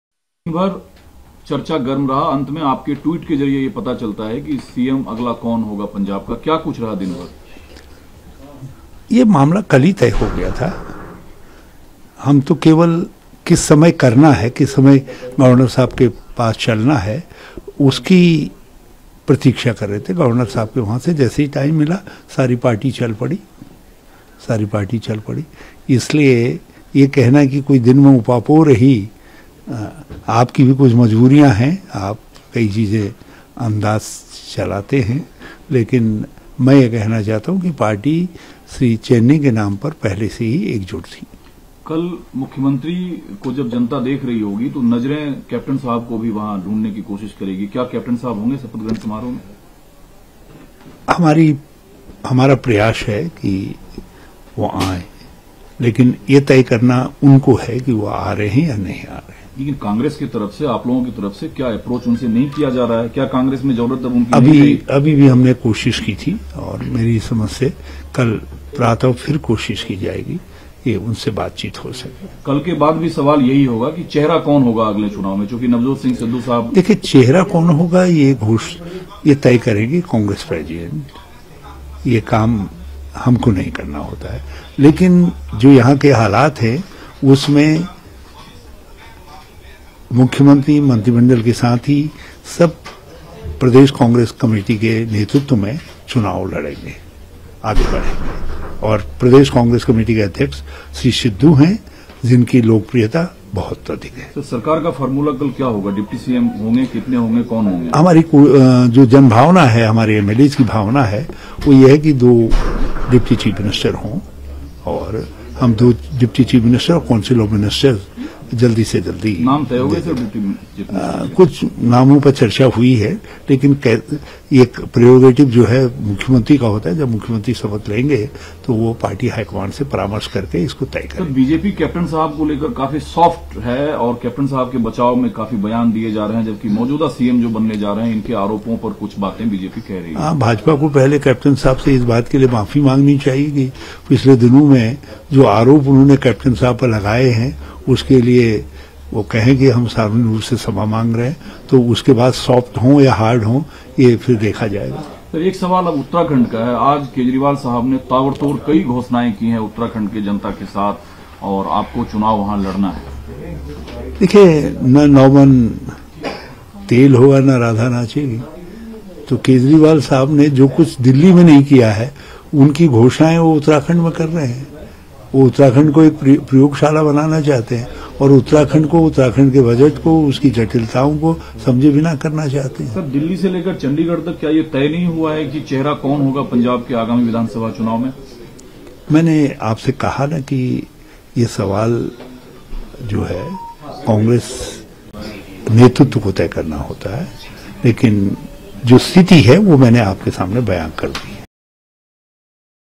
हरीश का सिद्धू के नेतृत्व में Punjab Elections लड़ने का बयान चौंकाने वाला : Jakhar | ETV Bharat
कांग्रेस नेता सुनील जाखड़ ने पंजाब विधानसभा का चुनाव नवजोत सिंह सिद्धू के नेतृत्व में लड़े जाने के कांग्रेस के वरिष्ठ नेता हरीश रावत के बयान को चौंकाने वाला बताया है. उन्होंने यह बातें अपने ट्वीट में कही हैं.